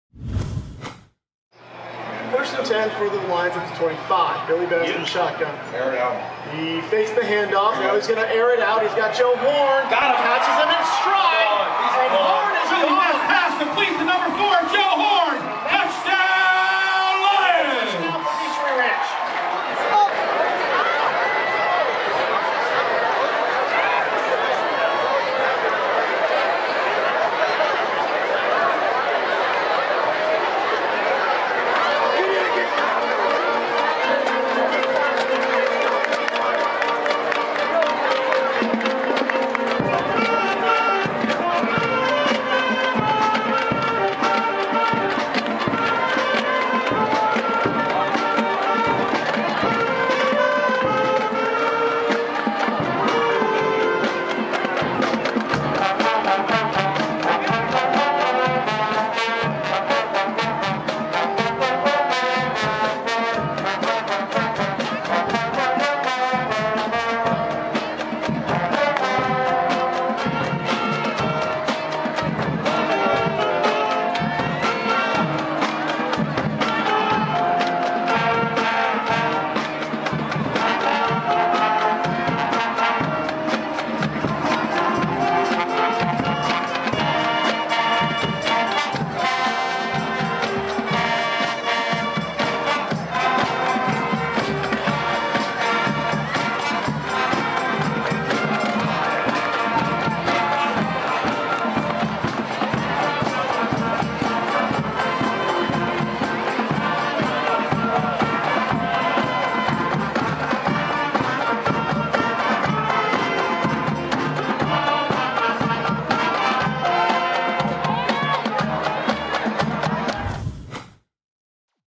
Peachtree Ridge vs. Norcross football/band 2011
This is a highlight video of football and band action at Peachtree Ridge High, Suwanee, GA, on Oct. 14, 2011. Story